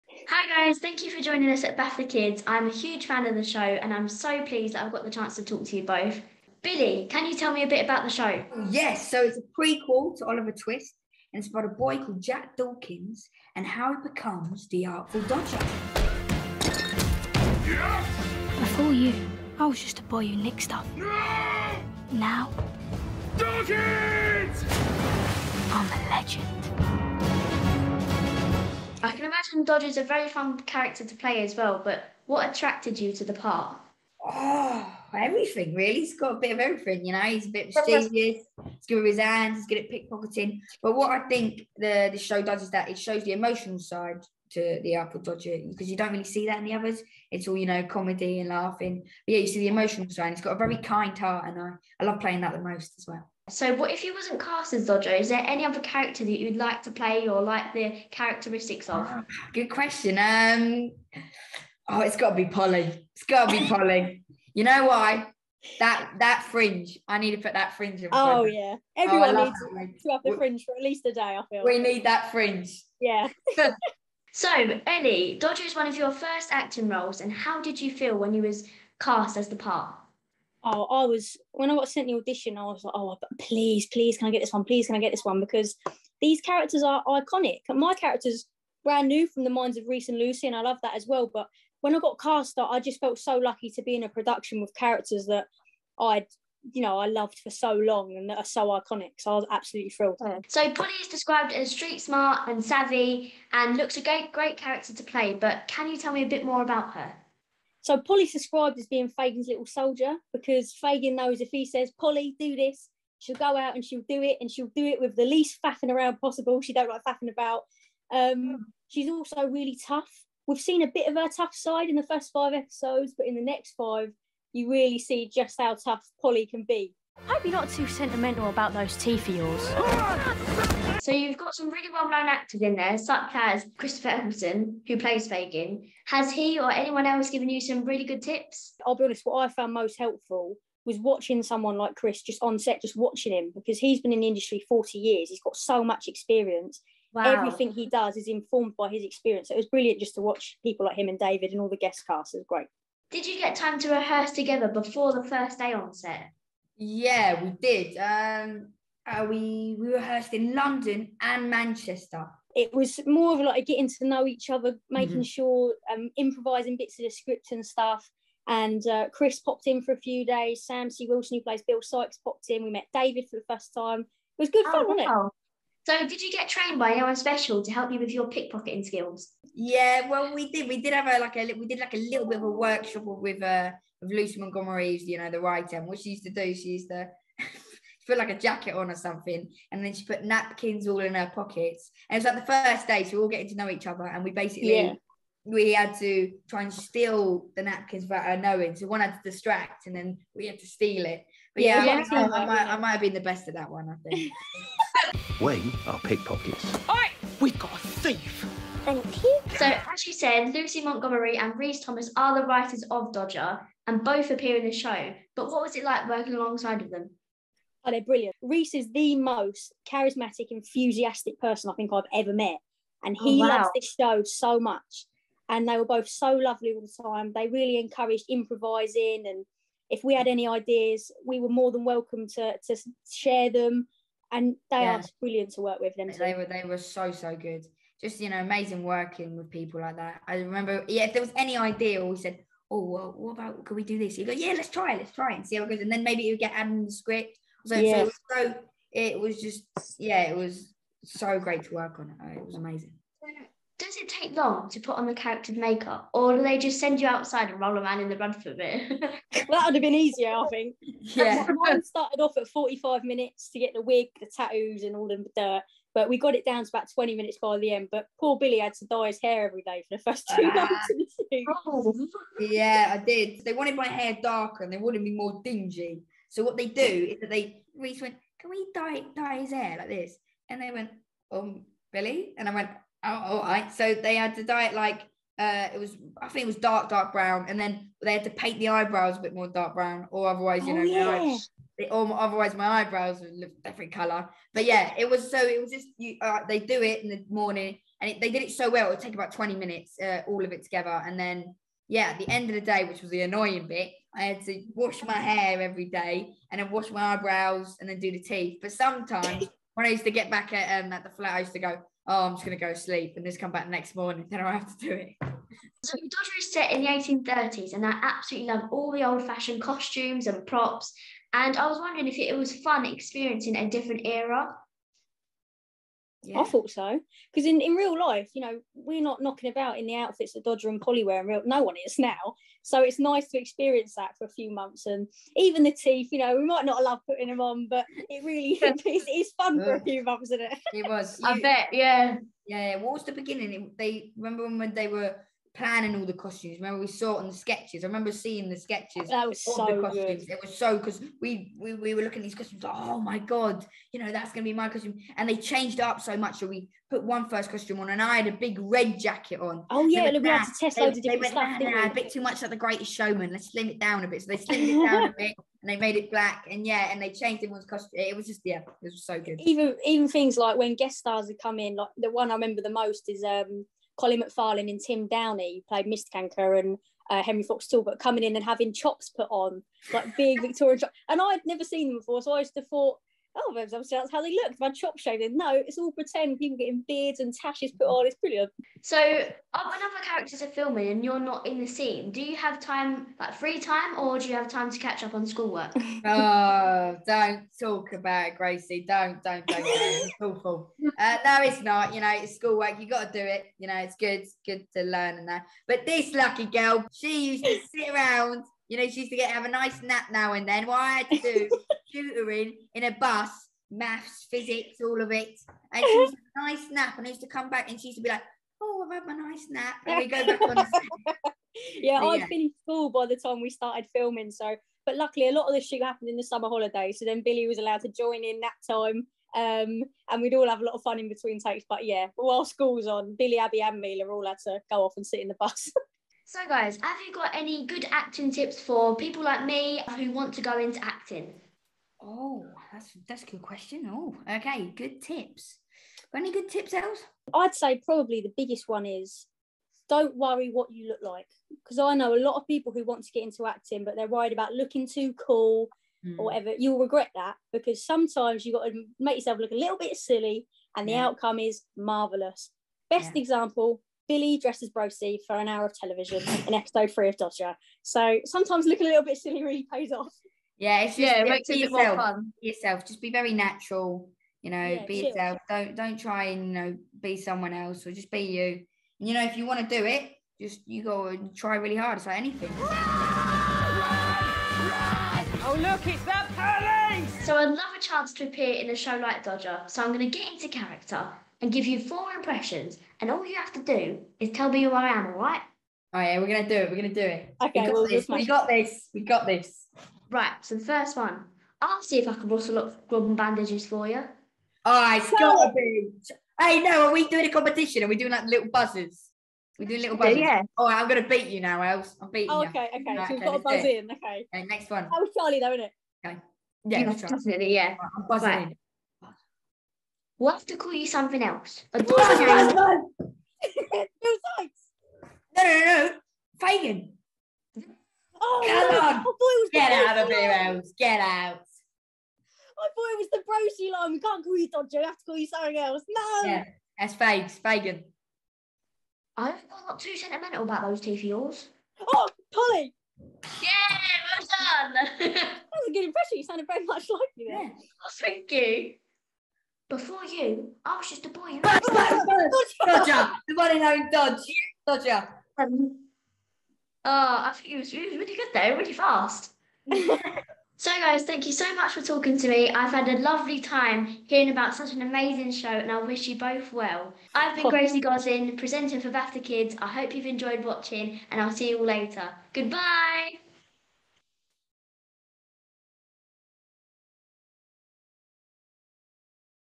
Young Presenter Gracie chats with the cast of Dodger
Young Presenter Gracie chats with the cast of Dodger: Billy Jenkins (Dodger) and Ellie-May Sheridan (Polly).  The young cast discuss pickpocketing workshops, the challenges of hair and make up for a Dickensian inspired drama and what they love about their characters.  Dodger is produced by Universal International Studios for the BBC.  It is on CBBC and BBC One on Sundays and available now on BBC iPlayer.